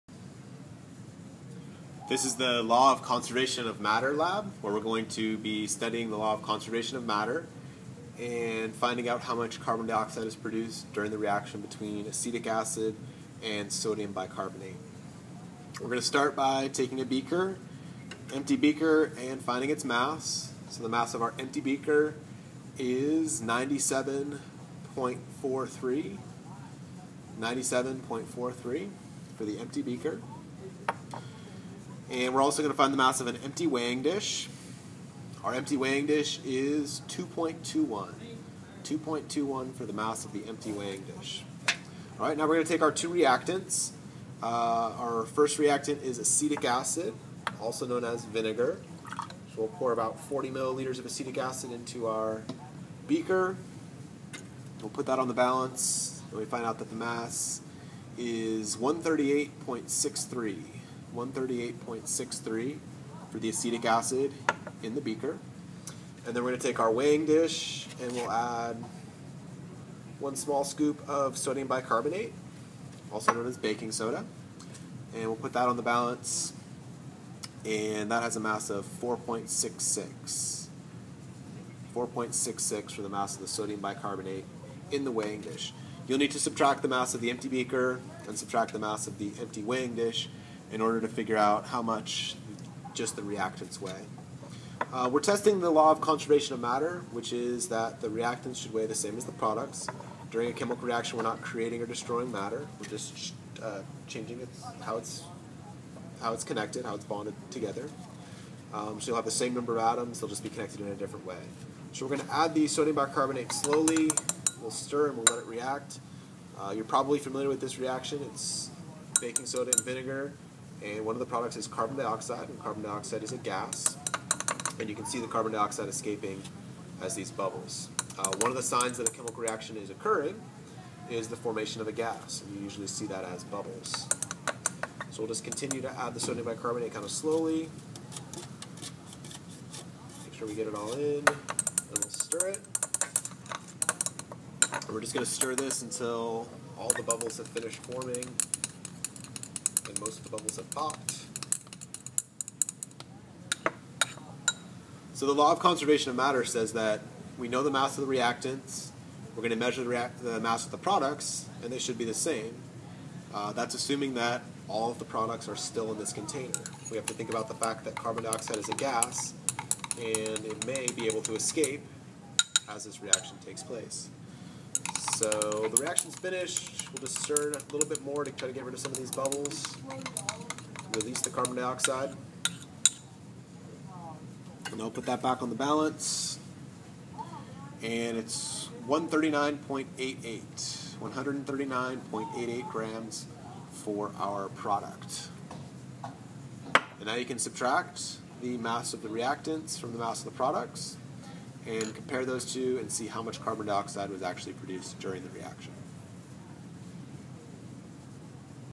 Witzgall Chemistry: Conservation of mass lab
Witzgall Chemistry: Conservation of mass lab.  Finding the mass of carbon dioxide produced when acetic acid reacts with sodium bicarbonate.